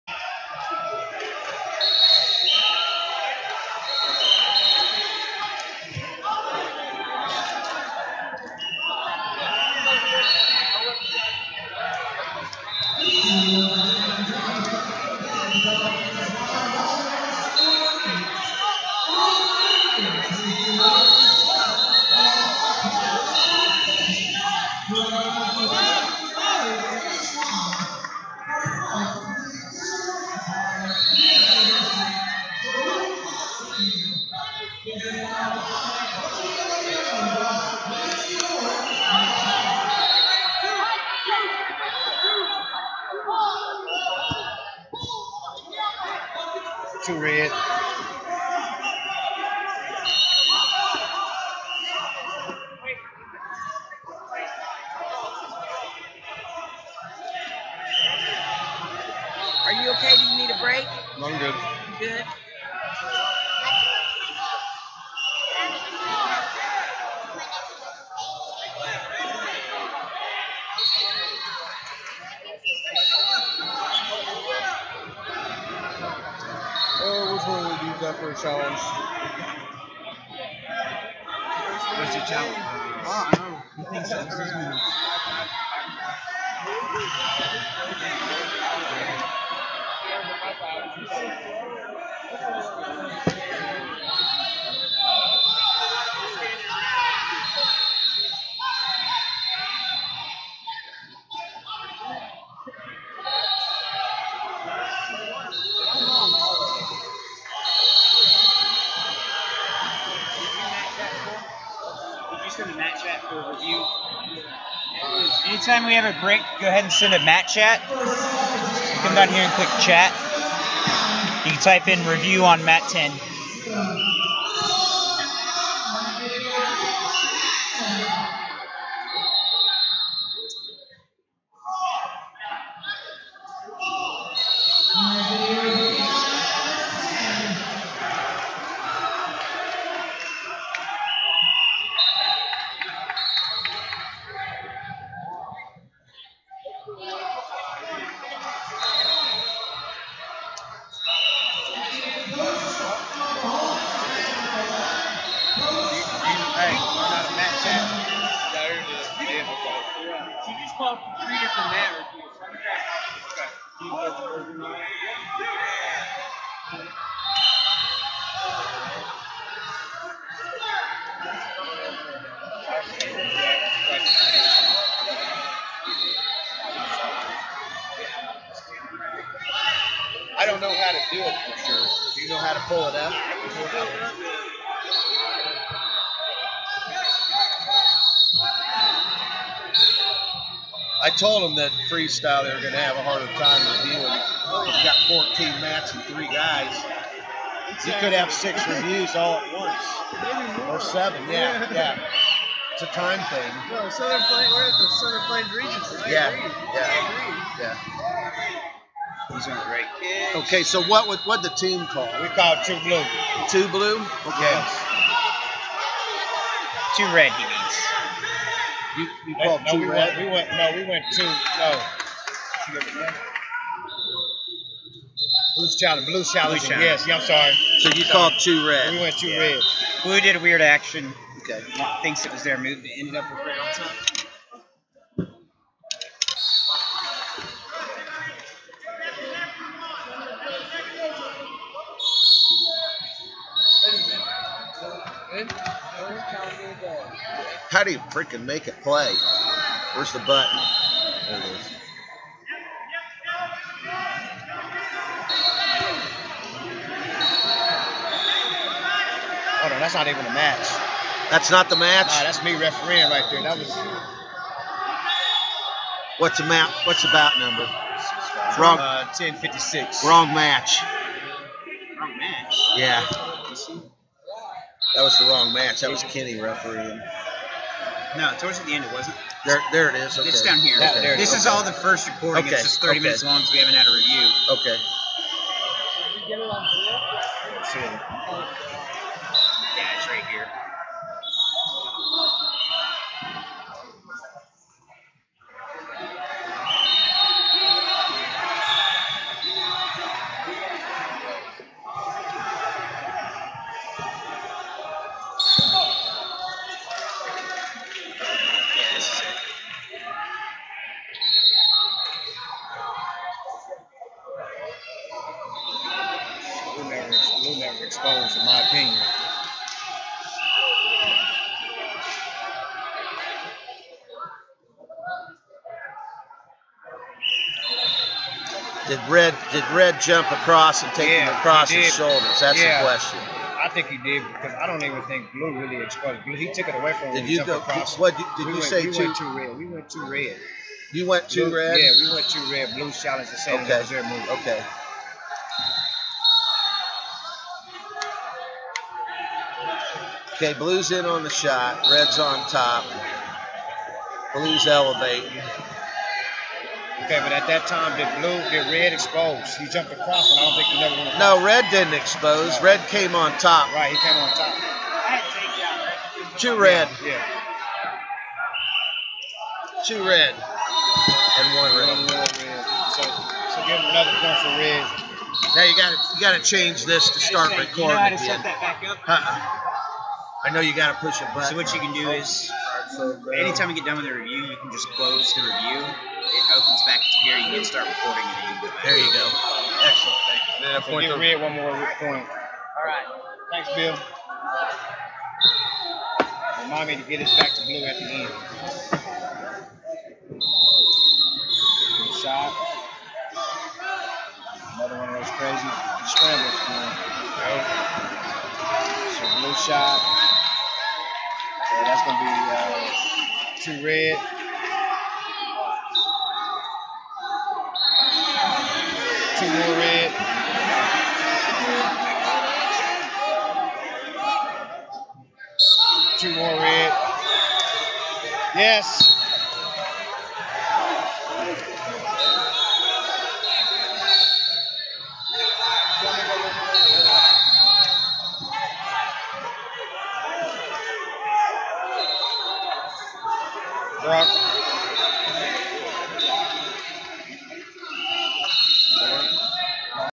132 Lbs Champ. Round 2 - Lukas Hainline, KS Vs Jackson Herman, TX 3a39
2022 Southern Plains Regional Championships - 132 Lbs Champ. Round 2 - Lukas Hainline, KS Vs Jackson Herman, TX 3a39